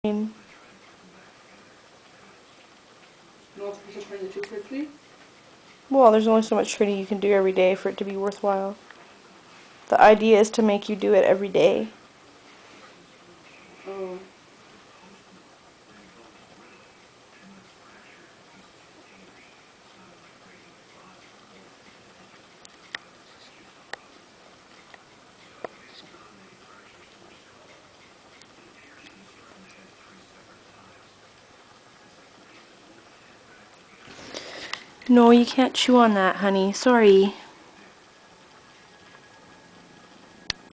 Delta on the laptop
Delta likes to sit on the keyboard.  That horrible grating voice is mine- I'm talking to my sister about the Nintendo DS game Brain Age. I didn't realize that the digicam I was using captures sound as well as image... whoops :P